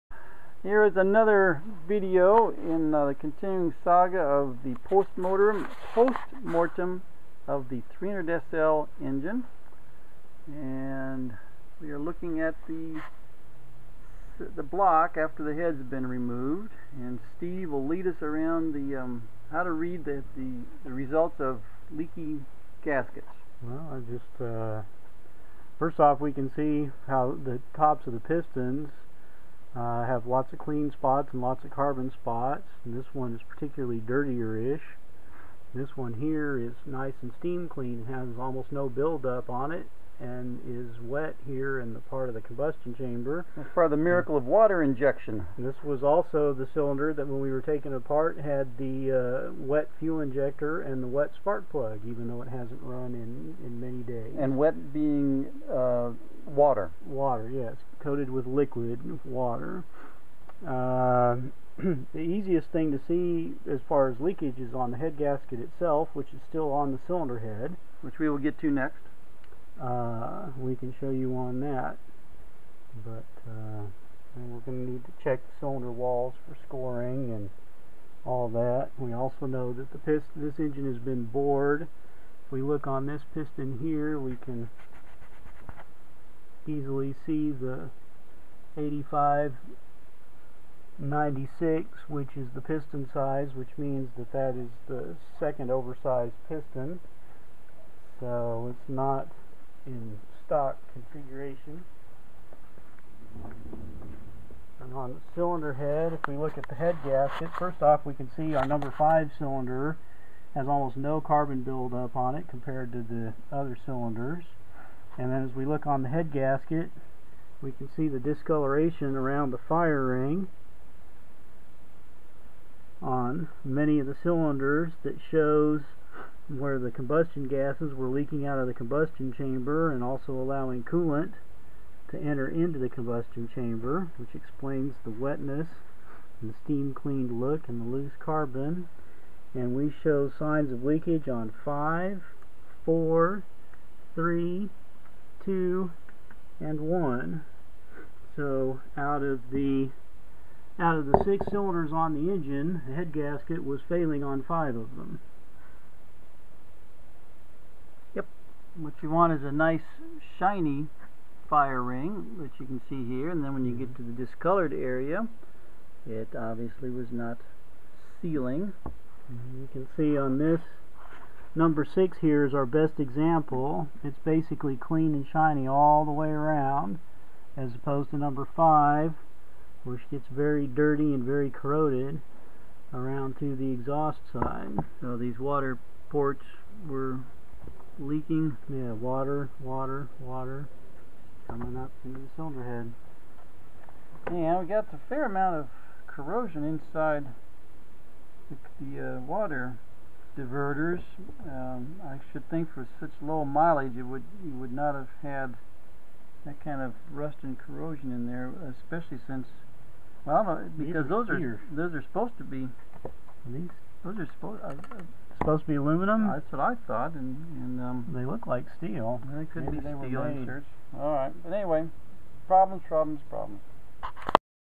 300SL Roadster cylinder head removed
The 300SL cylinder head is removed and reveals that the head gasket was leaking on 5 of the 6 cylinders.  Once the head is disassembled and cleaned we will determine the cause of the leakage.